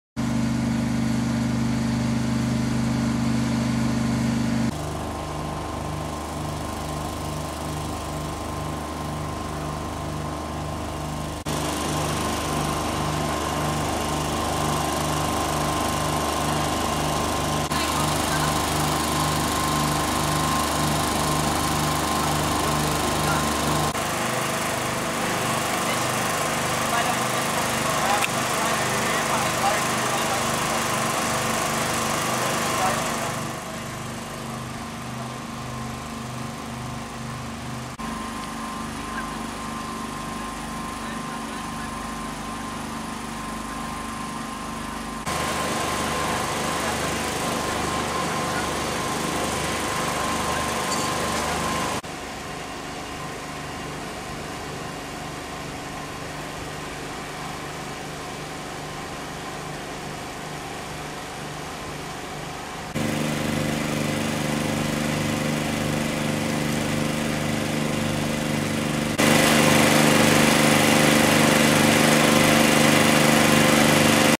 Radfahrer gegen Lieferwagen - schwerstverletzt in Köln-Kalk am 20.02.16
Ein Radfahrer prallte gegen einen Lieferwagen und verletzte sich dabei schwerst in Köln-Kalk am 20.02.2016